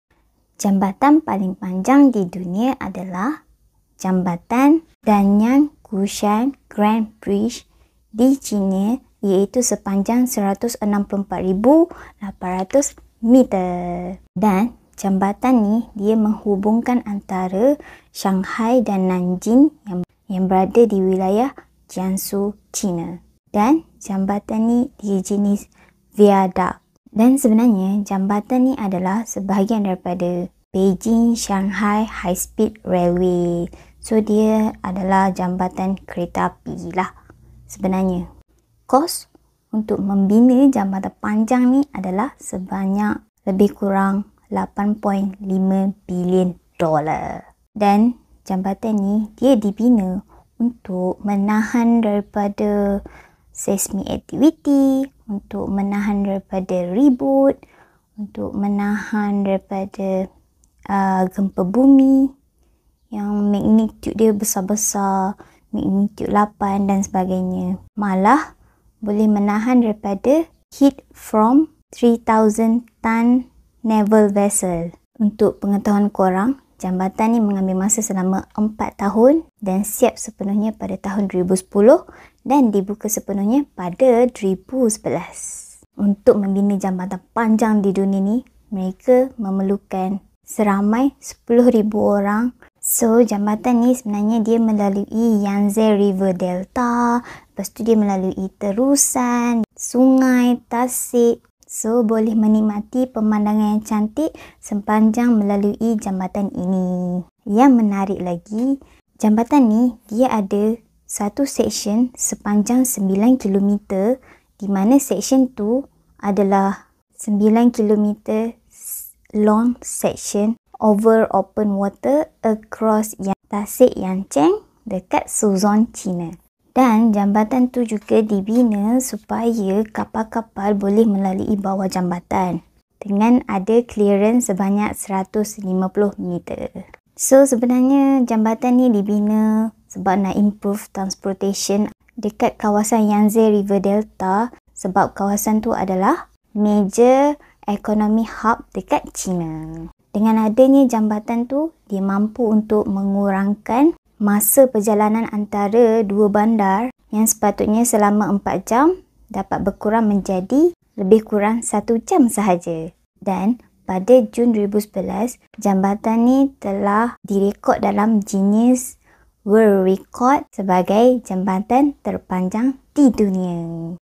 Jambatan Paling Panjang Di Dunia
Hai Assalamualakum semua!

Untuk video ini, wawa nak share tentang jambatan paling panjang di dunia.

Boleh dapatkan lebih banyak info daripada sumber-sumber ini:

1) Top 10: World’s longest bridges in the world - BBC Science Focus Magazine

2) The Ten Longest Bridges On Earth - Bridge Masters

3) Worlds Longest Bridge | Longest Bridge in the World | Live Science

5) The 10 Longest Bridges in the World | HowStuffWorks

8) A Compact Guide To Danyang-Kunshan Grand Bridge For Tourists

Terima kasih semua yang menonton video ini.
Harap dapat memberi manfaat serba sedikit.